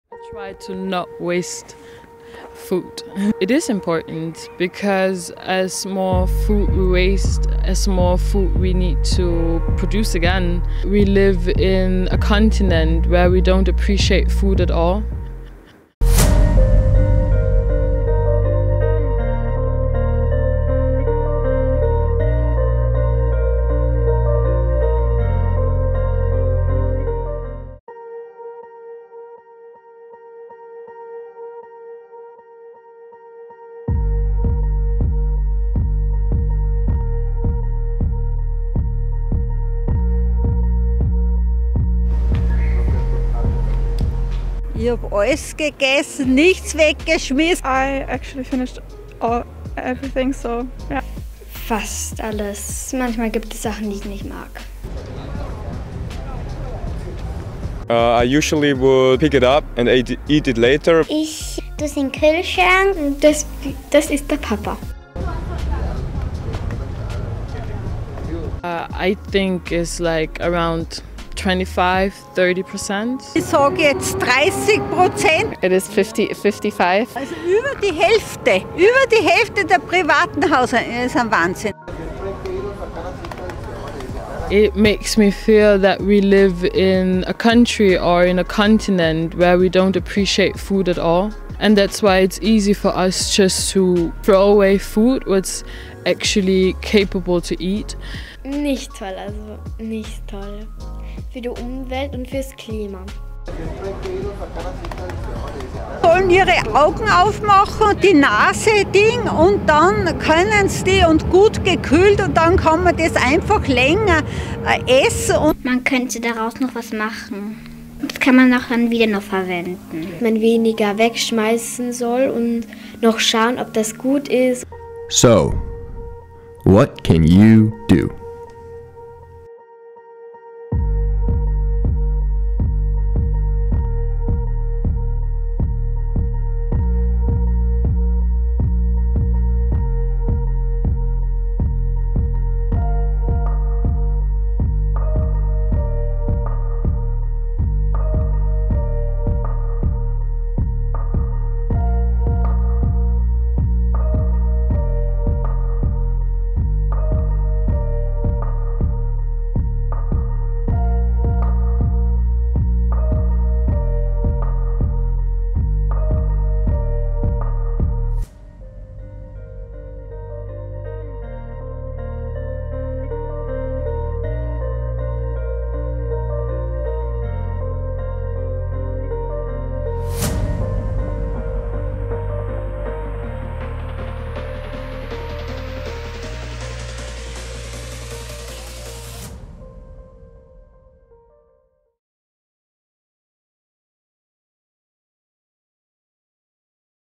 Wasted opportunities | BIP 2022
Diese Kurzdokumentation setzt sich intensiv mit dem Thema Food Waste auseinander. Als Teil des Blended Intensive Programs in Linz interviewten acht internationale Studierende Menschen aller Generationen in der Linzer Innenstadt. Was tust du gegen die Verschwendung von Essen?